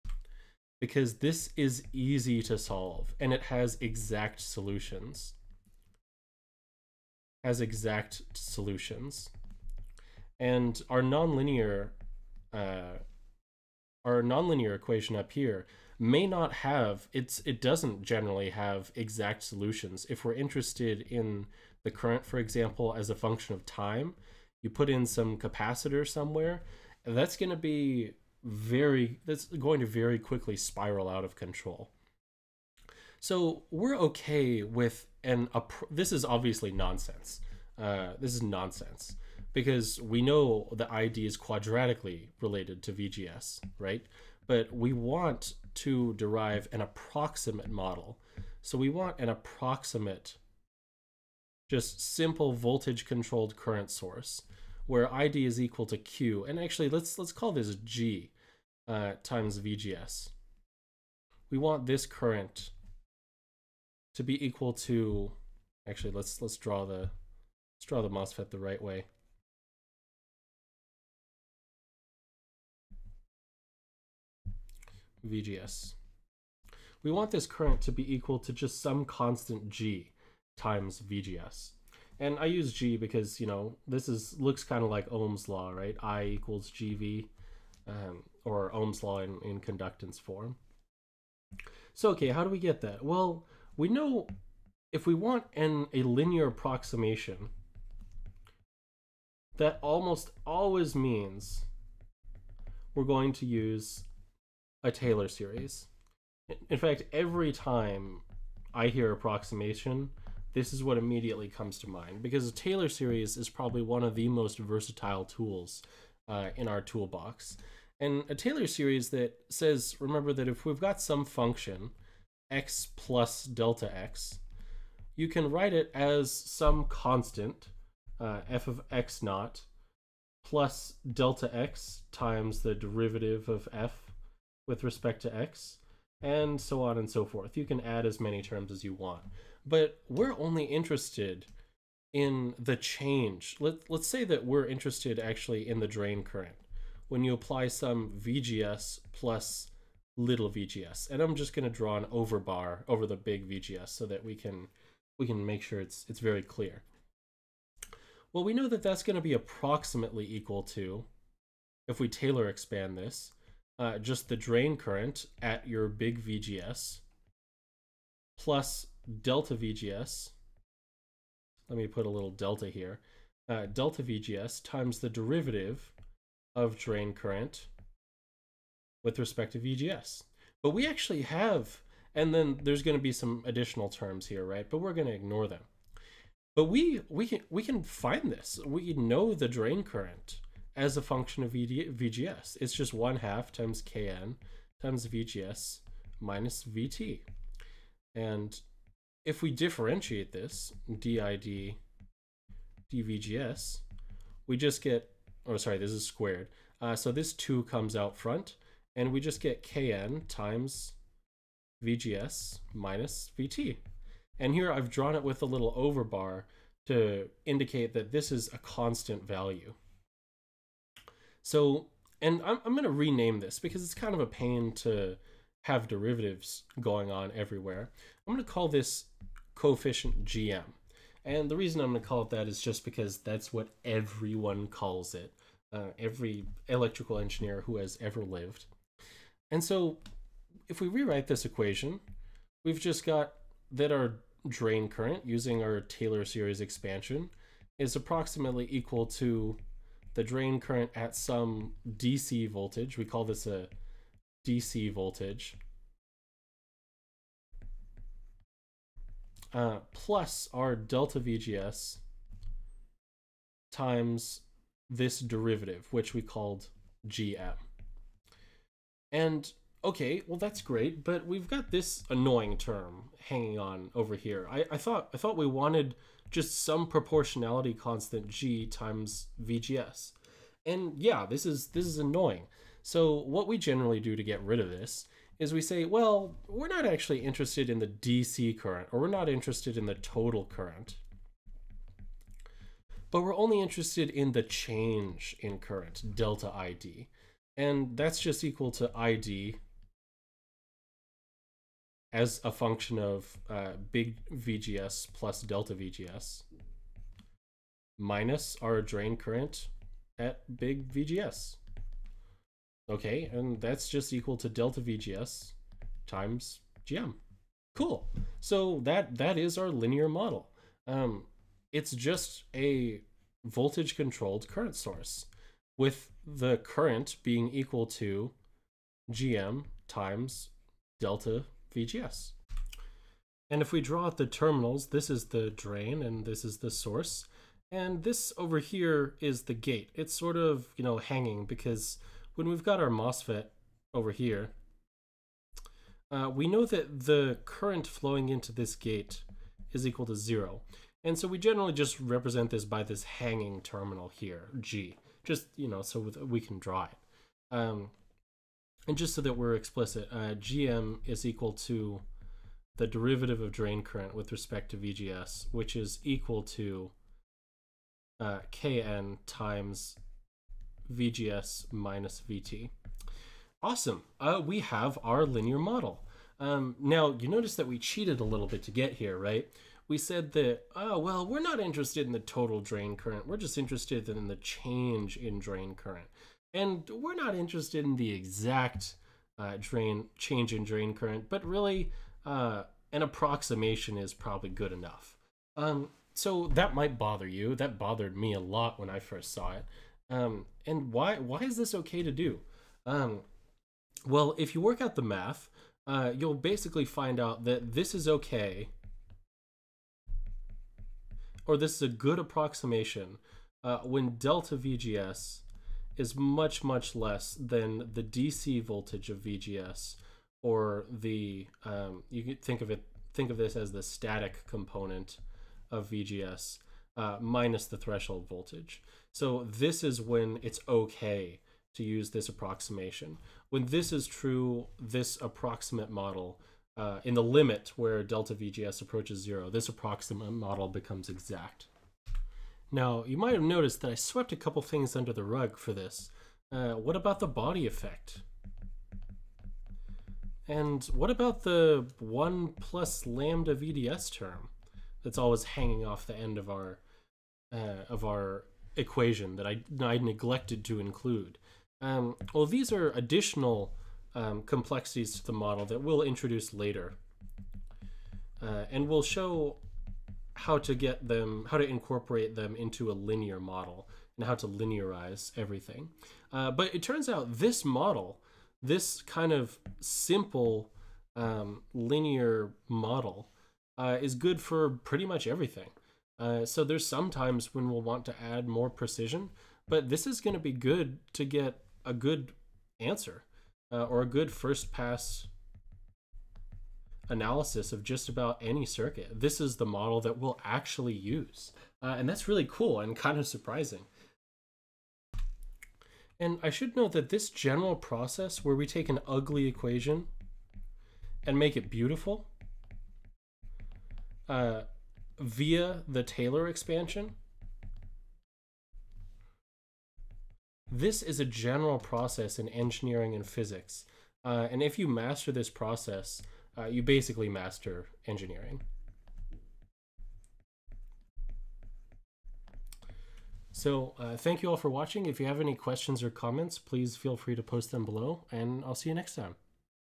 MOSFET Small Signal Model Explained
In this video, I derive and explain the MOSFET small-signal model: where it comes from, why we need it, and why it's actually awesome.